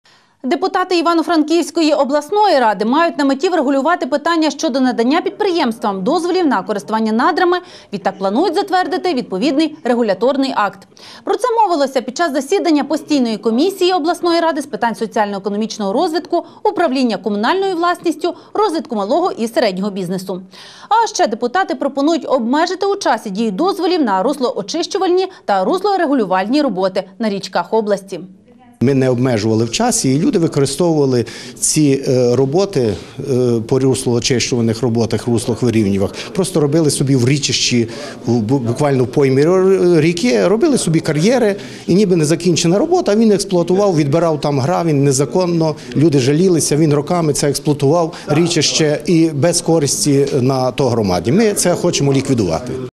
Засідання комісії з облради з питань соціального розвитку
Депутати Івано-Франківської обласної ради мають на меті врегулювати питання надання підприємствам дозволів на користування надрами, відтак, планують затвердити відповідний регуляторний акт. Про це мовилося під час засідання постійної комісії обласної ради з питань соціально-економічного розвитку, управління комунальною власністю, розвитку малого і середнього бізнесу. А ще депутати пропонують обмежити у часі дію дозволів на руслоочищувальні та руслорегулювальні роботи на річках області.